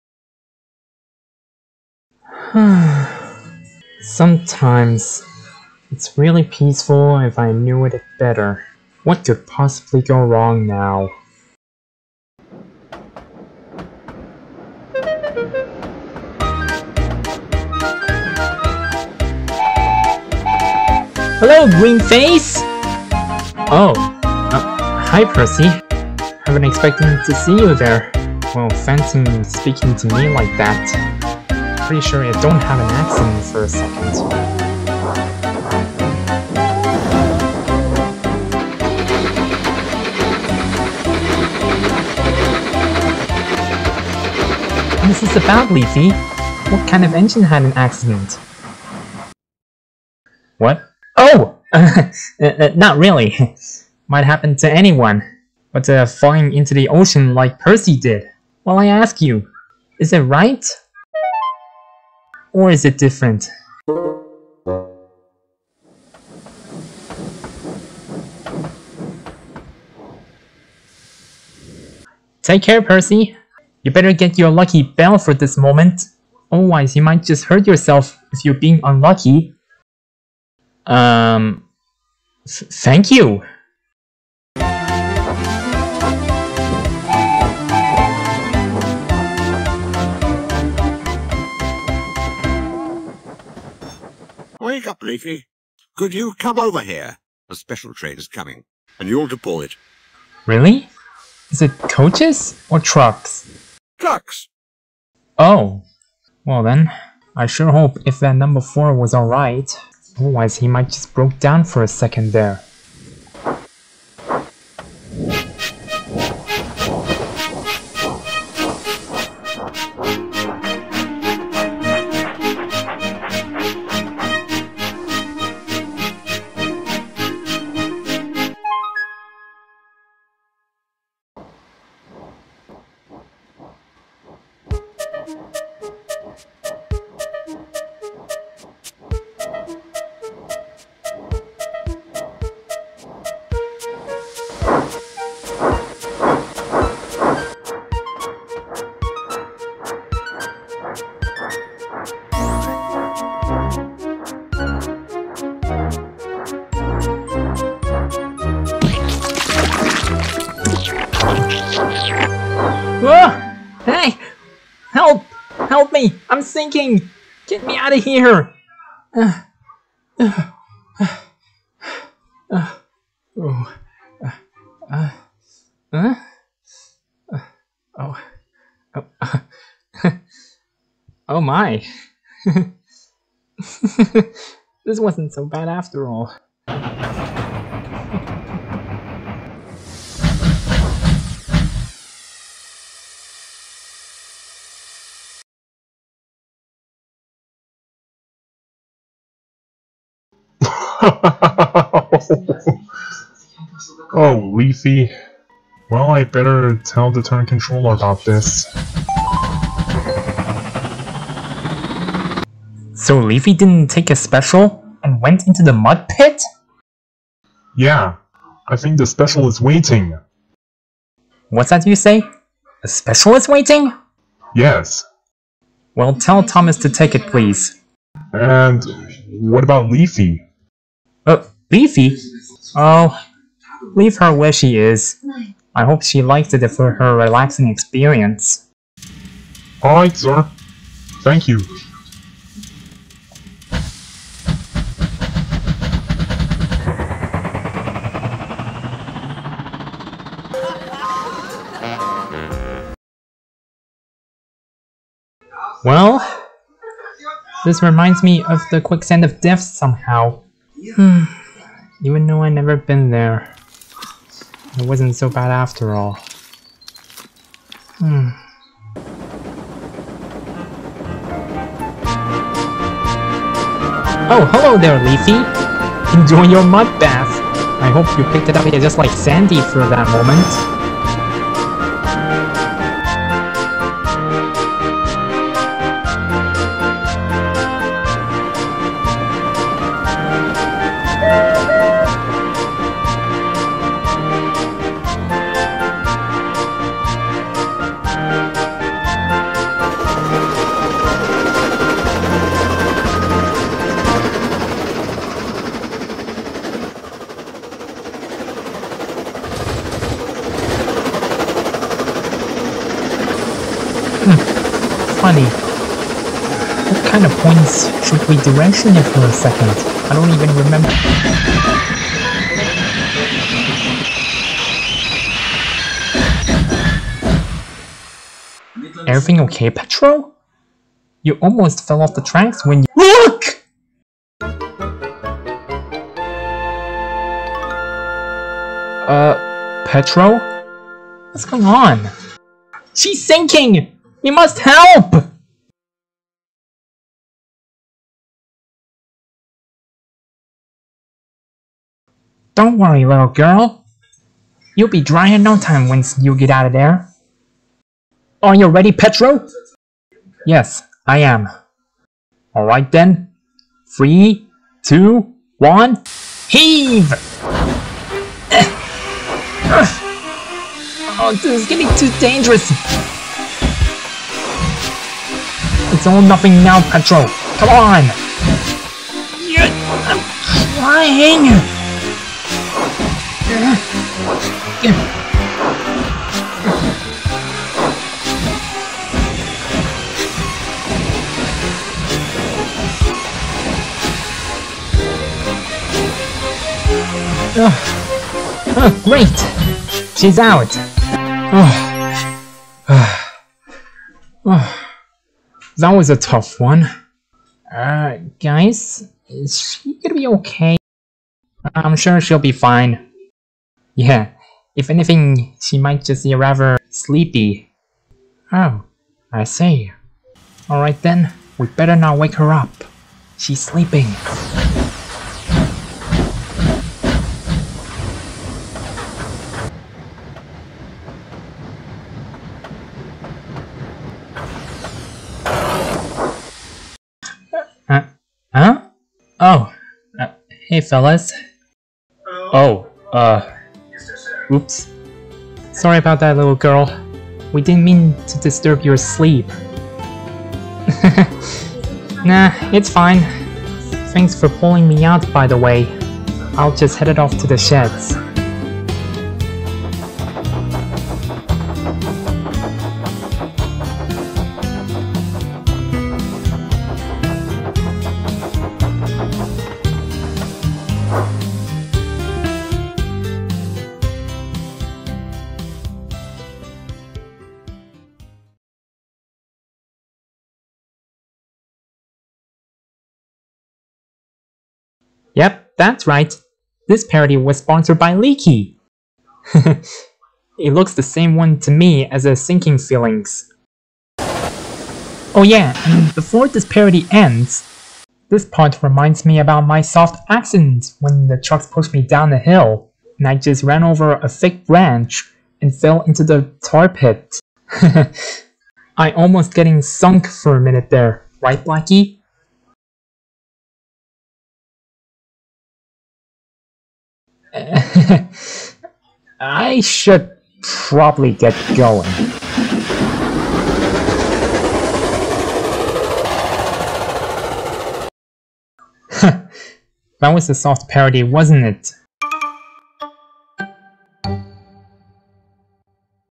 Leafy's Sinkful Day
Leafy was resting in a siding,
while she was interrupted with someone
then she had a work to do,
but then she accidentally went into the wrong direction
and straight into a soft pit of mud,
luckly she wasn't hurt but she was stuck in the pit,
then later that night, pedro max & peter come to lift leafy out of the pit,